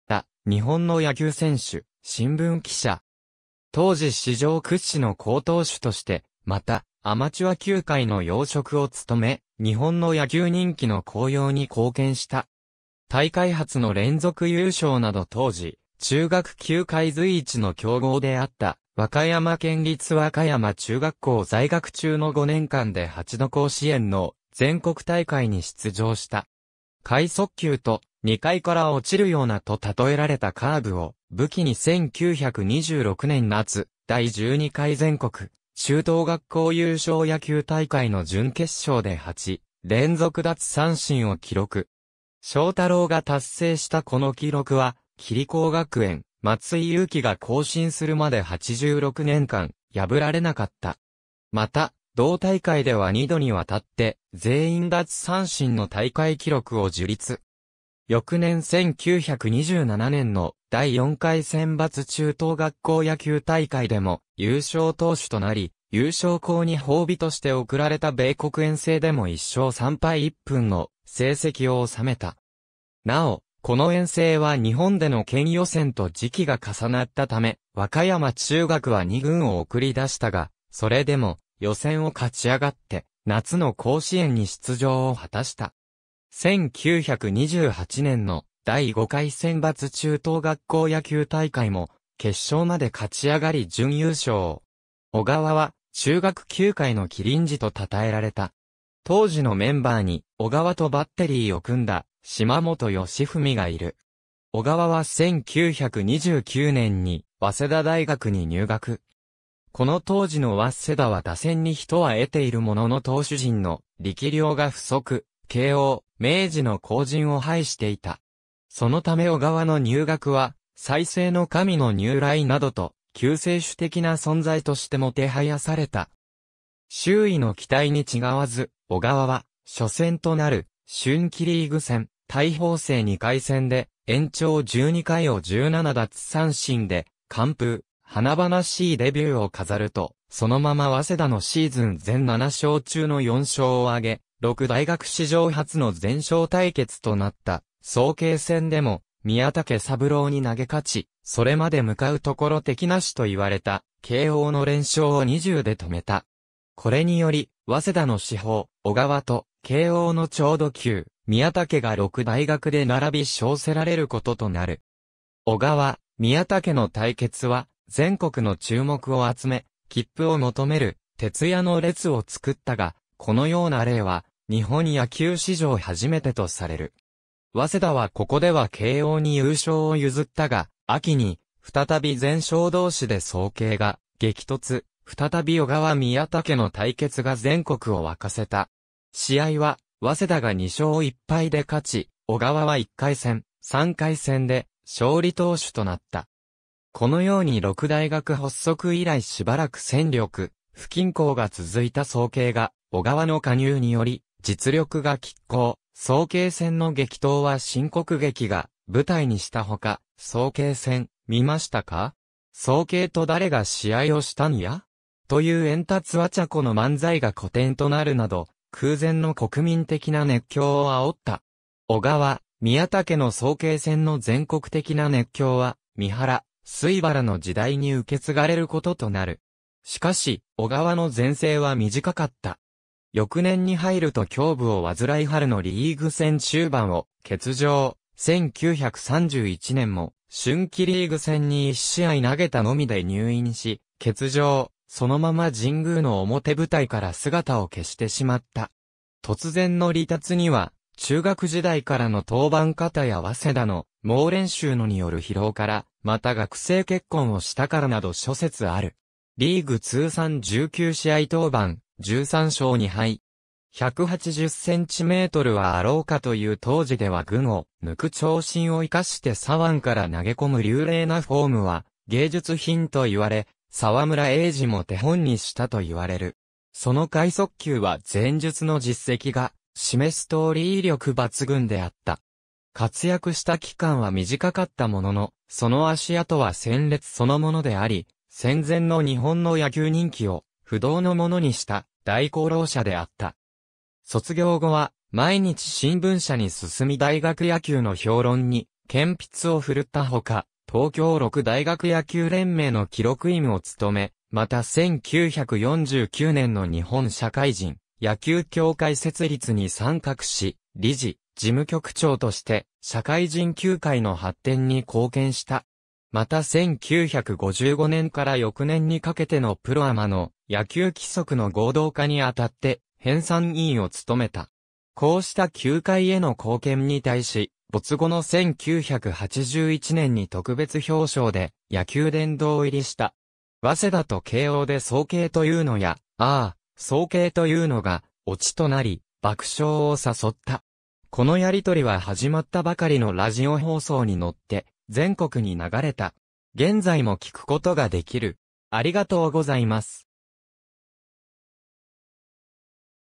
小川正太郎
小川 正太郎（おがわ しょうたろう、1910年4月1日 - 1980年10月27日）は、大正末期から昭和初期にかけて活躍した日本の野球選手、新聞記者。
当時史上屈指の好投手として、またアマチュア球界の要職を務め、日本の野球人気の高揚に貢献した。
大会初の連続優勝など当時中学球界随一の強豪であった和歌山県立和歌山中学校（現・和歌山県立桐蔭高等学校）在学中の5年間で8度甲子園の全国大会に出場した。
快速球と「2階から落ちるような」と喩えられたカーブを武器に1926年（大正15年）夏第12回全国中等学校優勝野球大会の準決勝で8連続奪三振を記録。
正太郎が達成した この記録は桐光学園・松井裕樹が更新するまで86年間破られなかった。
また同大会では2度にわたって「全員奪三振」の大会記録を樹立。
翌年1927年の第4回選抜中等学校野球大会でも優勝投手となり、優勝校に褒美として贈られた米国遠征でも1勝3敗1分の成績を収めた。
なお、この遠征は日本での県予選と時期が重なったため和歌山中学は二軍を送り出したが、それでも予選を勝ち上がって夏の甲子園に出場を果たした（1927年の第13回全国中等学校優勝野球大会）。
1928年の第5回選抜中等学校野球大会も決勝まで勝ち上がり準優勝。
小川は「中学球界の麒麟児」と讃えられた。
当時のメンバーに小川とバッテリーを組んだ島本義文（のち横浜高工→阪急）がいる。
小川は1929年に早稲田大学に入学（のち政治経済学部に進んだ）。
この当時の早稲田は打線に人は得ているものの投手陣の力量が不足、慶應・明治の後塵を拝していた。
そのため小川の入学は「再生の神の入来（にゅうらい）」などと救世主的な存在としてもてはやされた。
周囲の期待に違わず、小川は初戦となる春季リーグ戦、対法政2回戦で延長12回を17奪三振で完封、華々しいデビューを飾ると、そのまま早稲田のシーズン全7勝中の4勝を挙げ、六大学史上初の全勝対決となった早慶戦でも宮武三郎に投げ勝ち、それまで向かうところ敵無しといわれた慶應の連勝を20で止めた。
これにより「早稲田の至宝・小川」と「慶応の超ド級・宮武」が六大学で並び称せられることとなる。
小川・宮武の対決は全国の注目を集め、キップを求める徹夜の列を作ったが、このような例は日本野球史上初めてとされる。
早稲田はここでは慶應に優勝を譲ったが、秋に再び全勝同士（慶應8勝、早稲田5勝（対立教2回戦と明治戦を残していた）で早慶が激突、再び小川－宮武の対決が全国を沸かせた。
試合は早稲田が2勝1敗で勝ち、小川は1回戦・3回戦で勝利投手となった。
このように六大学発足以来しばらく戦力不均衡が続いた早慶が小川の加入により実力が拮抗、早慶戦の激闘は新国劇が舞台にした他、「早慶戦、見ましたか？」「早慶と誰が試合をしたんや？」というエンタツ・アチャコの漫才が古典となるなど、空前の国民的な熱狂を煽った。
小川・宮武の早慶戦の全国的な熱狂は三原・水原の次代に受け継がれることとなる。
しかし、小川の全盛は短かった。
翌年に入ると胸部を患い春のリーグ戦中盤を欠場、1931年も春季リーグ戦に1試合投げたのみで入院し欠場、そのまま神宮の表舞台から姿を消してしまった。
突然の離脱には中学時代からの登板過多や早稲田の猛練習のによる疲労から、また学生結婚をしたからなど諸説ある。
リーグ通算19試合登板、13勝2敗。
180cmはあろうかという当時では群を抜く長身を生かして左腕から投げ込む流麗なフォームは「芸術品」といわれ、沢村栄治も手本にしたといわれる。
その快速球は前述の実績が示すとおり威力抜群であった。
活躍した期間は短かったものの、その足跡は鮮烈そのものであり、戦前の（そして戦後に続く）日本の野球人気を不動のものにした大功労者であった。
卒業後は毎日新聞社に進み大学野球の評論に健筆を揮ったほか、東京六大学野球連盟の記録員を務め、また1949年の日本社会人野球協会設立に参画し、理事・事務局長として社会人球界の発展に貢献した。
また1955年から翌年にかけてのプロ・アマの野球規則の合同化にあたって編纂委員を務めた。
こうした球界への貢献に対し、没後の1981年に特別表彰で野球殿堂入りした。
^ 「早稲田と慶応で早慶というのや」「ああ、そーけー」というのがオ...